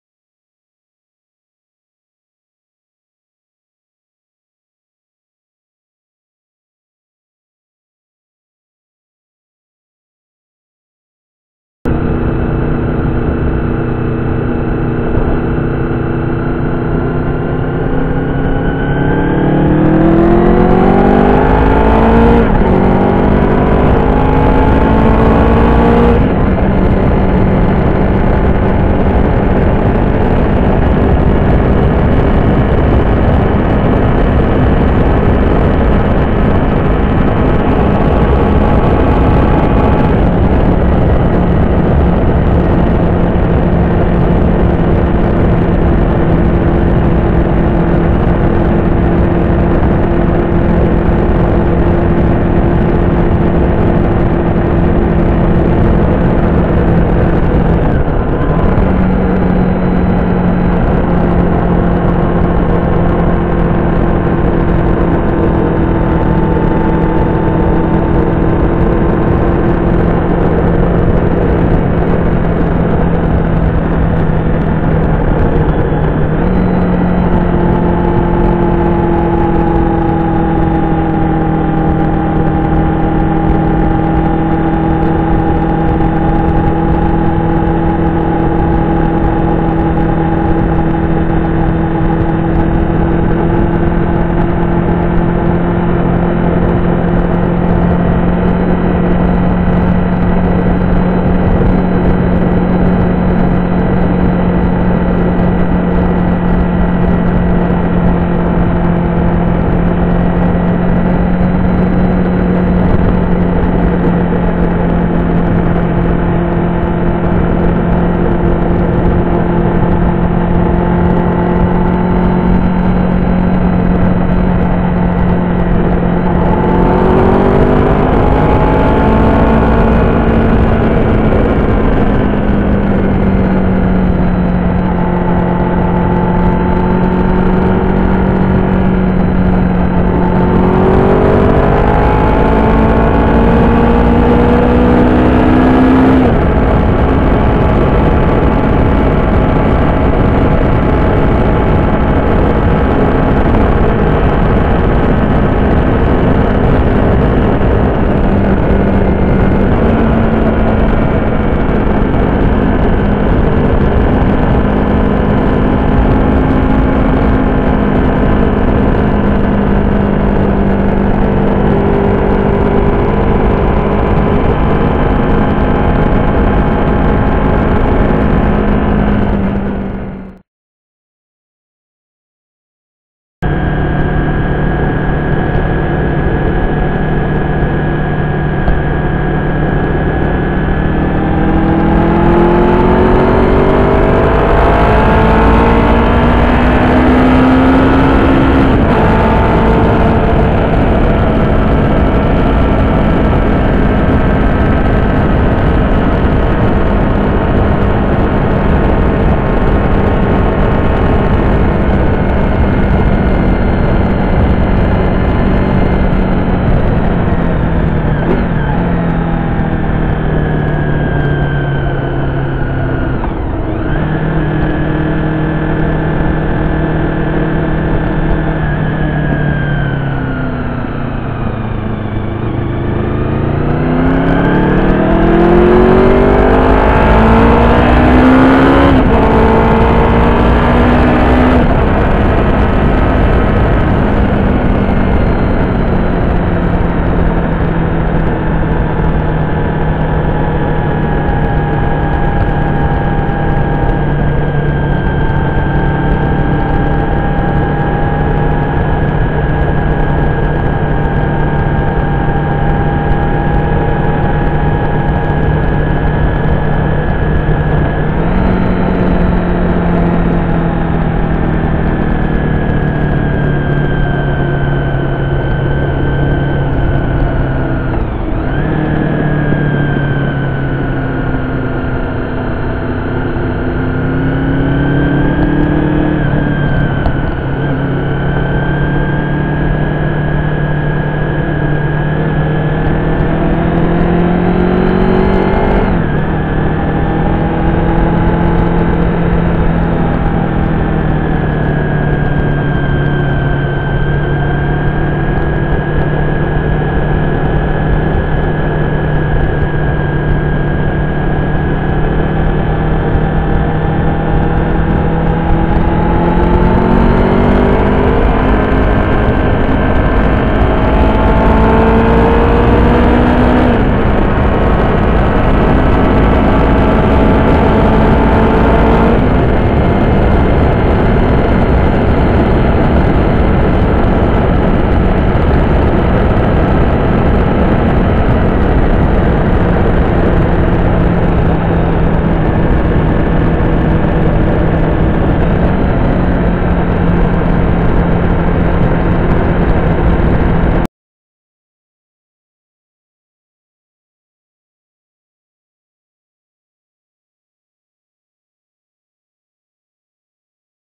Multiple Overtakes on a 1997 VFR750
A couple of examples of multiple vehicles overtakes on my way to and from work riding my Honda VFR750. I am NOT an expert, this is NOT an instructional video. I welcome feedback. Apologies if the quality is not good, it's my first attempt. Camera is tank mounted using a fat gecko camera mount.